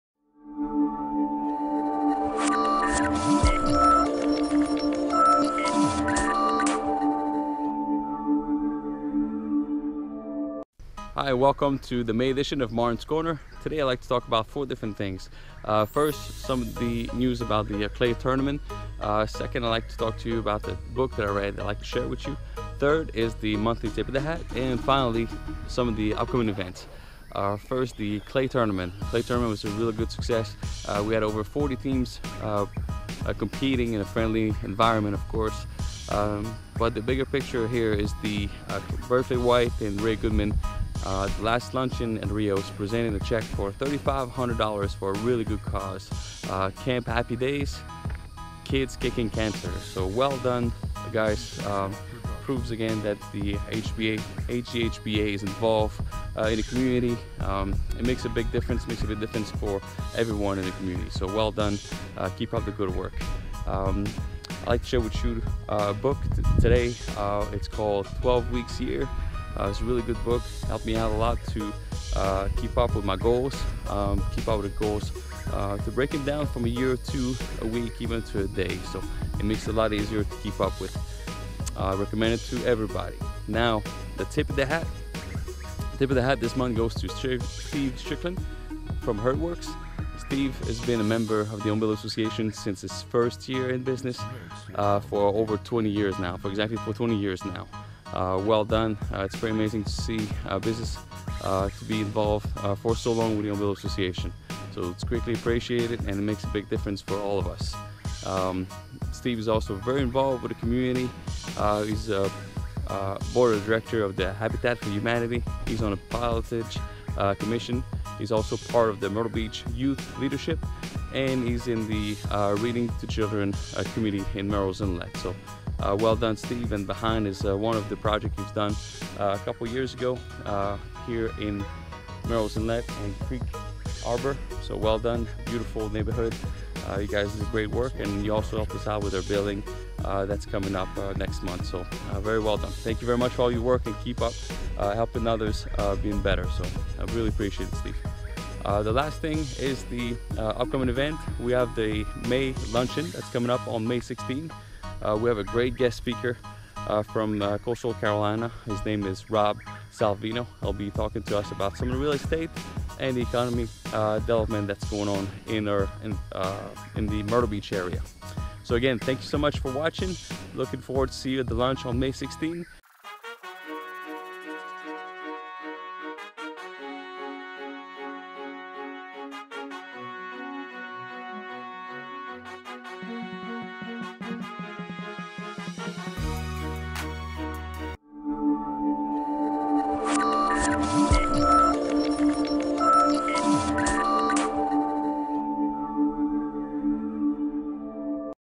Martin's Corner May Edition 2017. Monthly edition HGHBA. Tip of the Hat. Upcoming Events
Martin's Corner presents:

1) Clay Tournament
2) Book Recommendation
3) Tip Of The Hat
4) Upcoming Events

Martin Pettigrew
HGHBA President